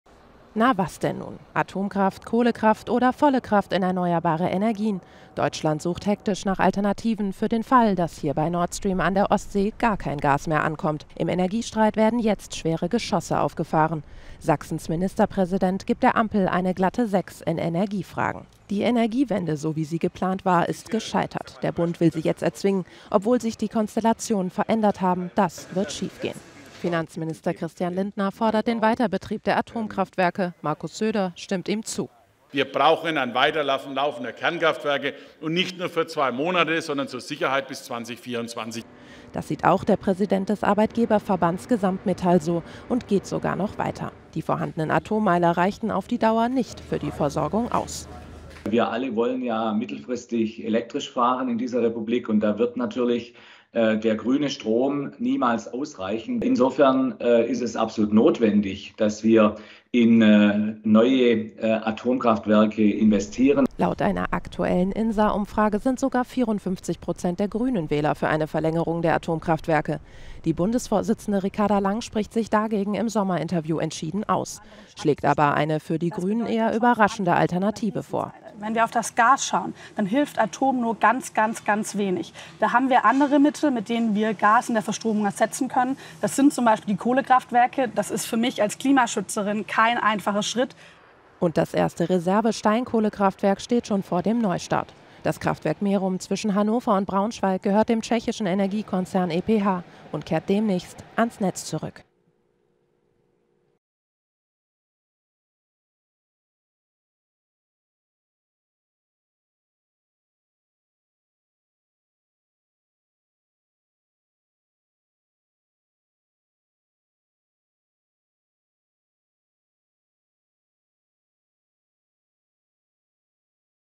KRISE IN DEUTSCHLAND: Energiewende? Die vernichtende Kritik von Michael Kretschmer
Sachsens Ministerpräsident Michael Kretschmer erklärt die Energiewende für gescheitert. Gleichzeitig fordert er die Laufzeiten der AKW zu verlängern. Die Grünen-Vorsitzende Ricarda Lang erteilt dem eine klare Absage. Doch auch die Industrie macht Druck.